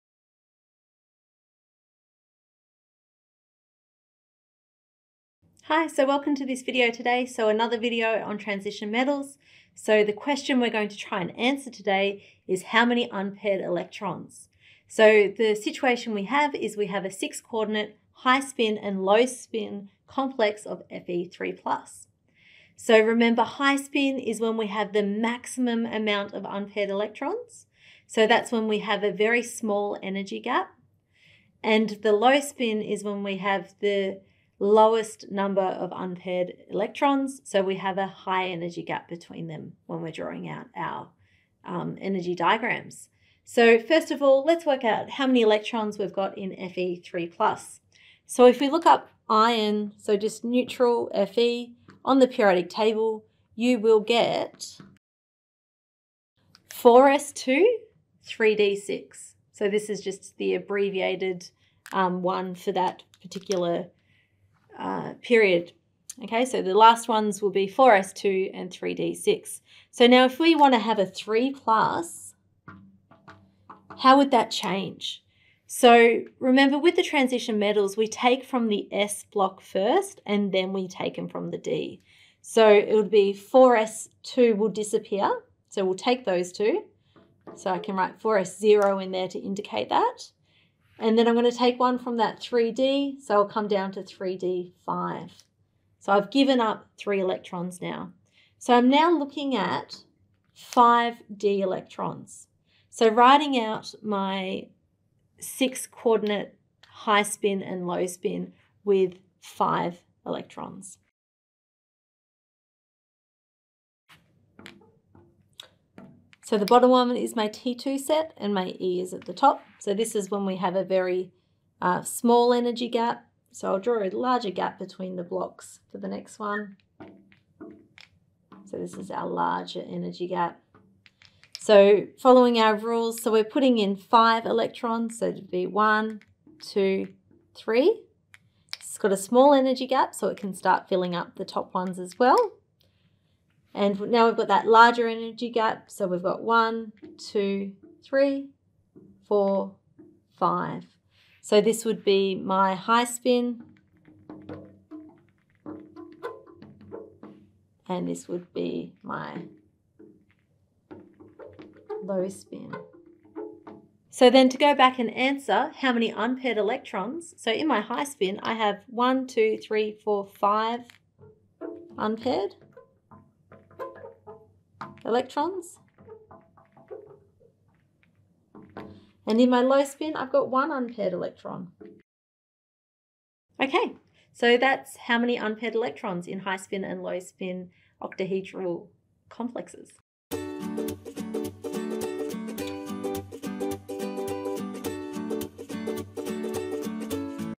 high-spin and low-spin transition metal complex example
This lightboard video goes through an example on - How many unpaired electrons would exist in a 6-coordinate high-spin
and low-spin complex of Fe3+ ?